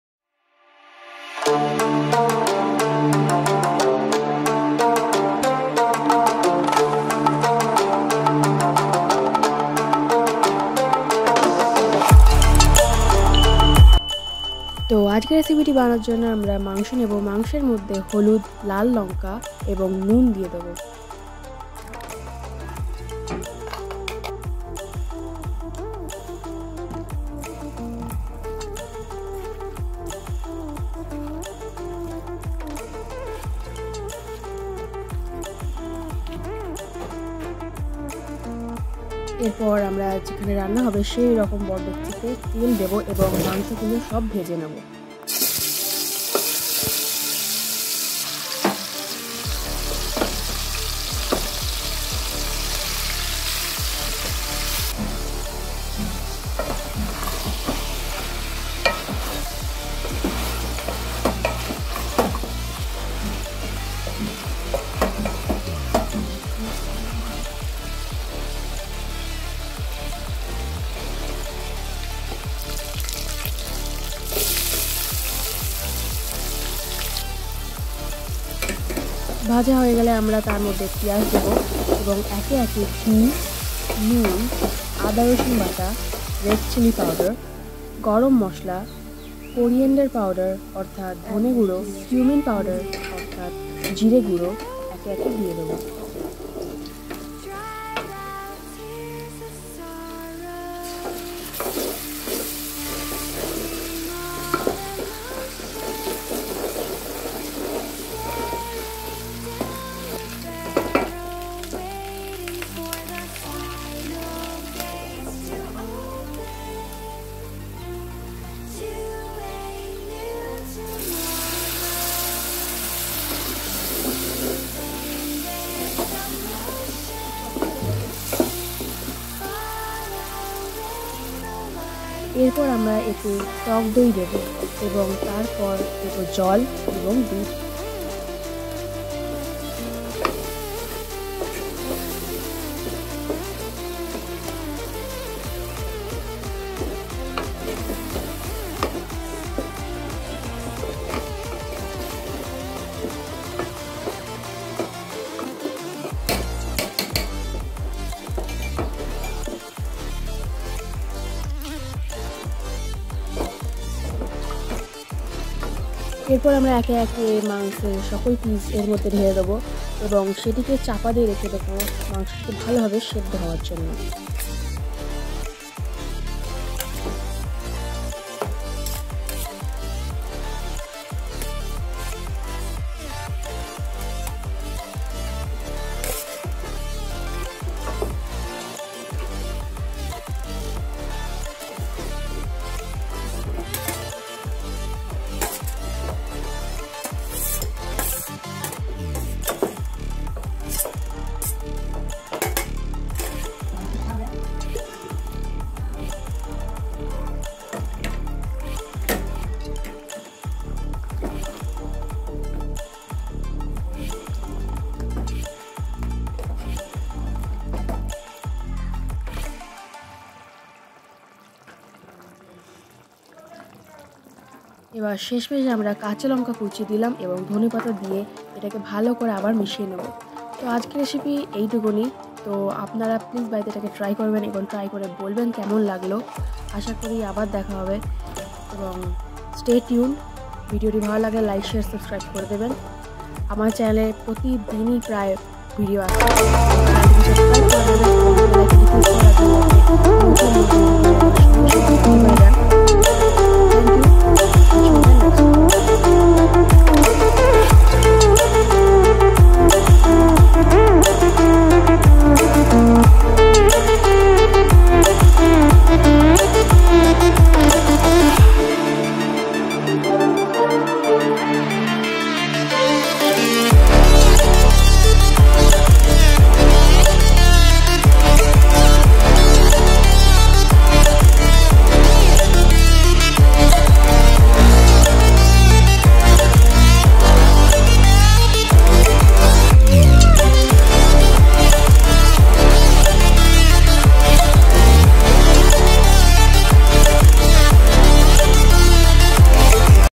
কাশ্মীরি চিকেন মসলা | KASHMIRI CHICKEN MASALA RECIPE | CHICKEN MASALA CURRY | Piya Pal
KASHMIRI CHICKEN MASALA RECIPE | CHICKEN MASALA CURRY | CHICKEN MASALA | THE KITCHEN

Ingredients ~

TURMERIC POWDER
SALT
MUSTARD OIL
ONION
DESI GHEE
SALT
GARAM MASALA
CORAINDER POWDER
CUMIN POWDER
WATER
CURD
MILK
FRIED CHICKEN
CORAINDER LEAVES